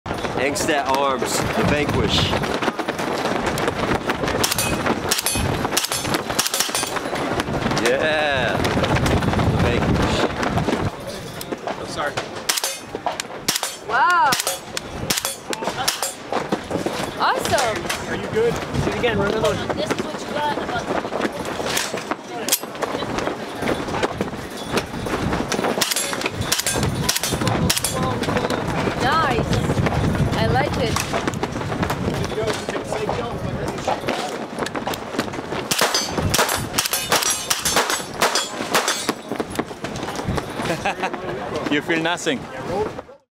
Orin Julie - Michael Paa - Mark Mauer with the Angstadt Arms Vanquish 9mm Suppressor
Showcasing the Angstadt Arms Vanquish Baffleless suppressor at Shot Show Range Day 2023.